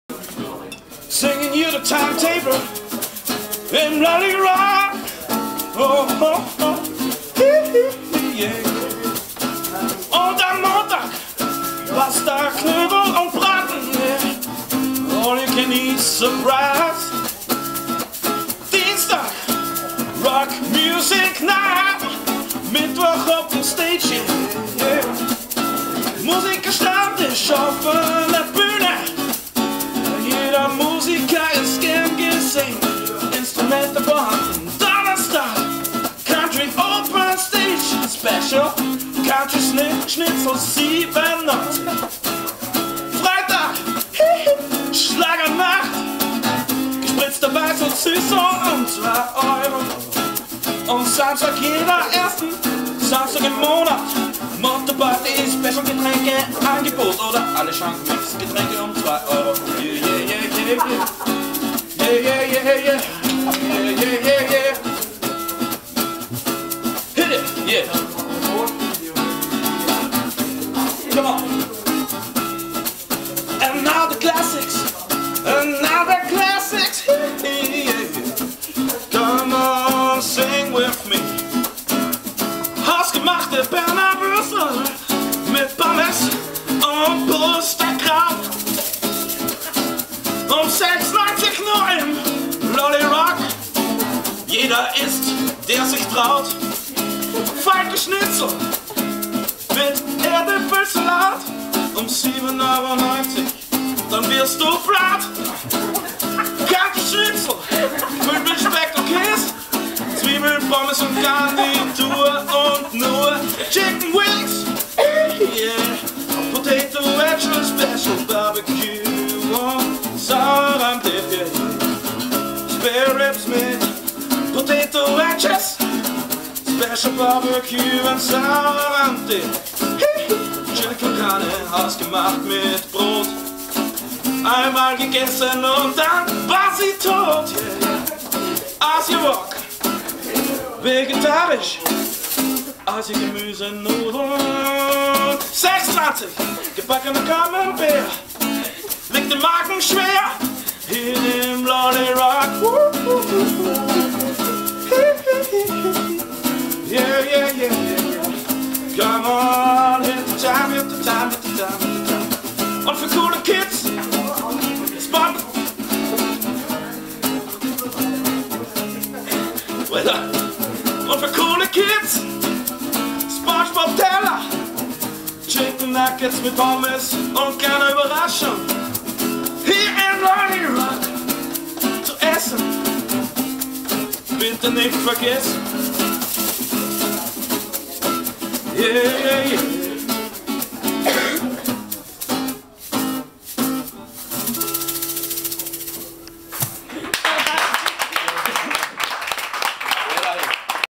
Weltneuheit - das gab es nur im LolliRock!
Tagesangebote und Speisekarte gesungen von Peter